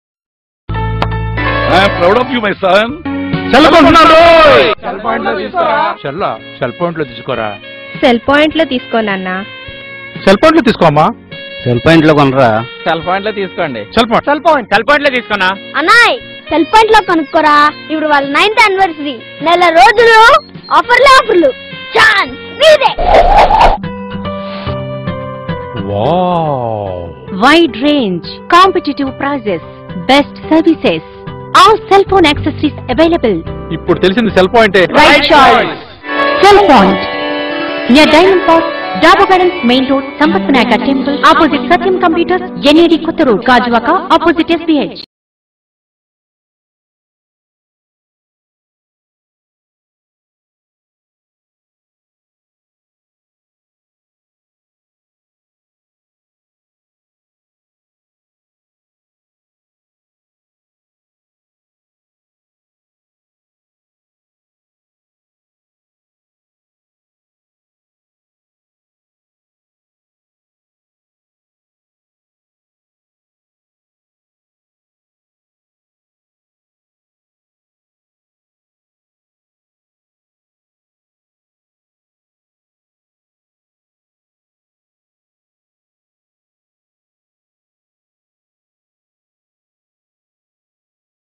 Cell Point - TV Commercial
Cell Point is one of the fastest-growing retail mobile chain industry in Andhra Pradesh, with more than 100+ stores.